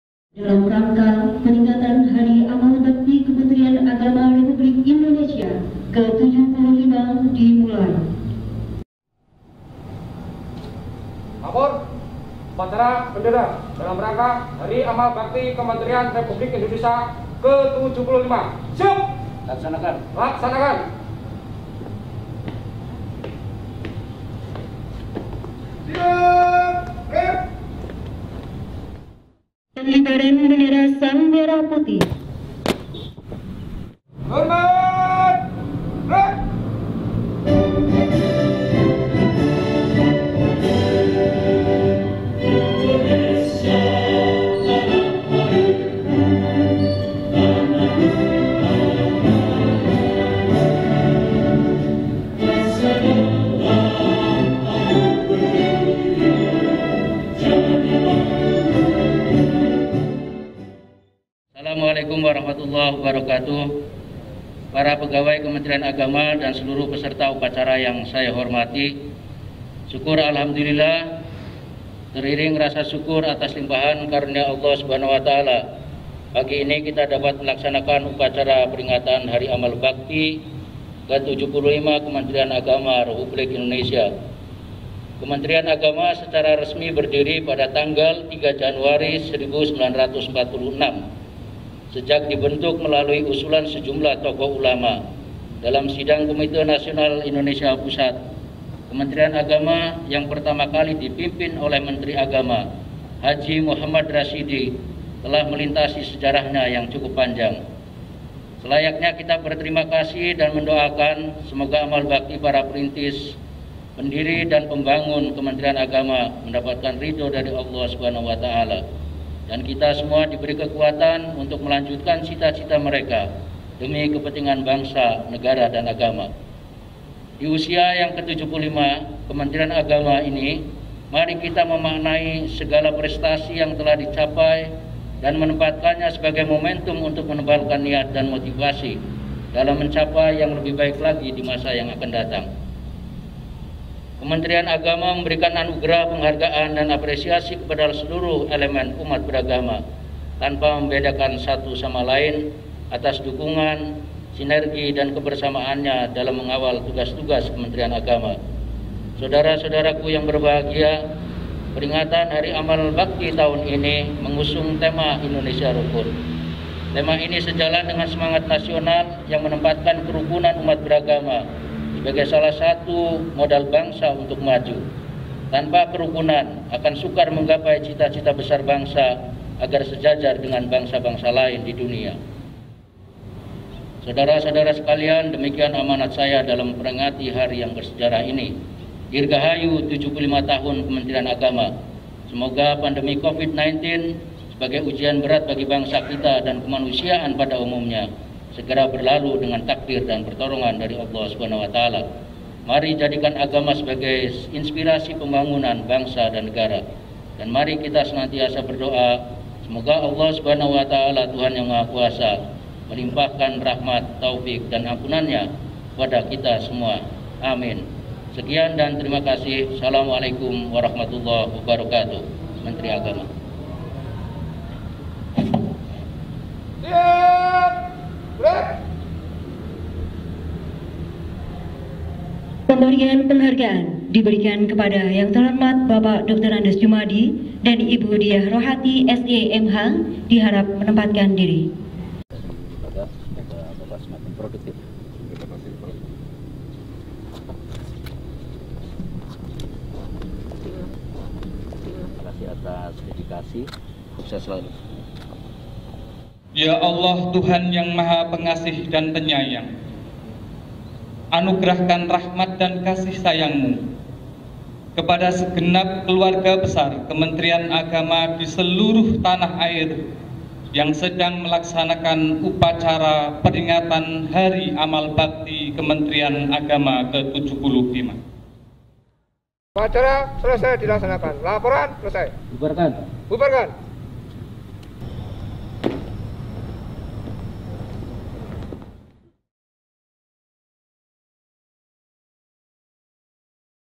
Upacara Hari Amal Bakti Ke-75 Kementerian Agama RI
Assalamu'alaikum

Sivitas Akademik IAIN Salatiga mengadakan upacara peringatan Hari Amal Bakti (HAB) ke-75 dengan menerapkan protokol kesehatan.

Pada kesempatan itu, Rektor IAIN Salatiga, Prof. Dr. Zakiyuddin Baidhawy, M.Ag membacakan amanat Menteri Agama RI, Yaqut Cholil Qoumas.

Menurut Menteri Agama dalam amanatnya, tema HAB 2021 Indonesia Rukun sejalan dengan semangat nasiomal yang menempatkan kerukunan umat beragama sebagai salah satu modal bangsa untuk maju.

@Kemenag RI @Pendis Channel